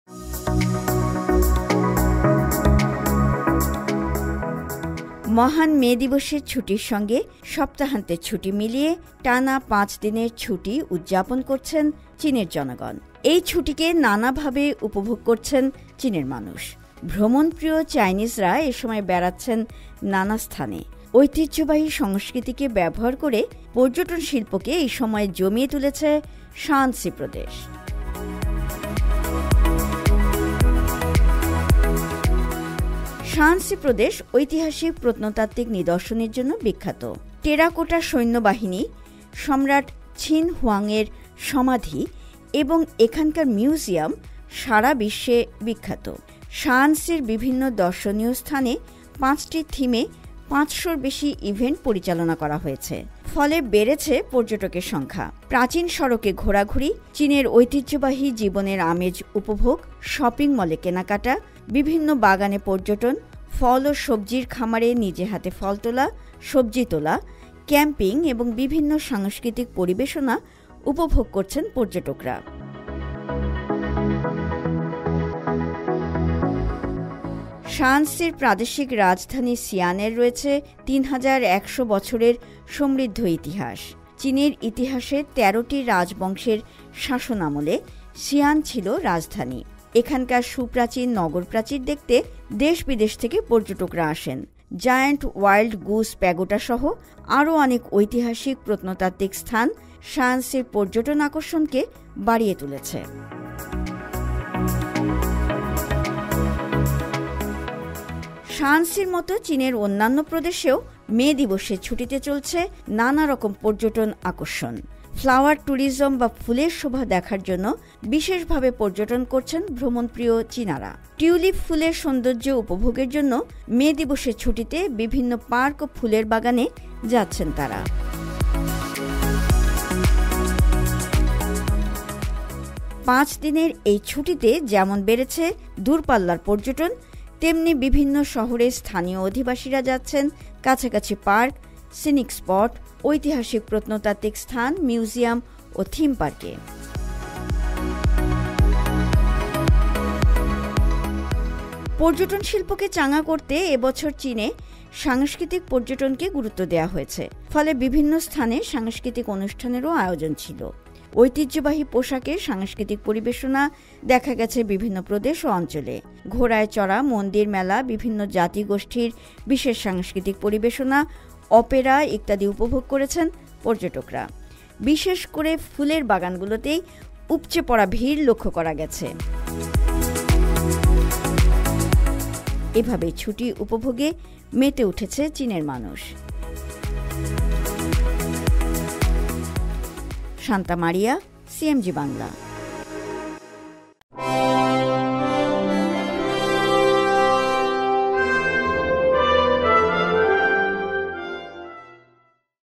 চীনে ঐতিহ্যবাহী সংস্কৃতিকে ব্যবহার করে পর্যটন শিল্প || CMG BANGLA
চীনে মে দিবসের ছুটিতে পর্যটন বেড়েছে। কয়েক দিনের ছুটি হওয়ায় দেশের বিভিন্ন প্রদেশের পর্যটন কেন্দ্রগুলোকে প্রস্তুত করা হয়েছে পর্যটকদের জন্য। এসব প্রদেশের মধ্যে শায়ানসি প্রদেশ একটি। পর্যটন শিল্পকে চাঙা করতে এই প্রদেশে বিভিন্ন রকম ব্যবস্থা গ্রহণ করা হয়েছে। ঐতিহ্যবাহী সংস্কৃতিকে ব্যবহার করে কীভাবে এই প্রদেশ পর্যটন শিল্পকে জমিয়ে তুলেছে তা থাকছে এই প্রতিবেদনে।

16A Shijingshan Road, Beijing, China. 100040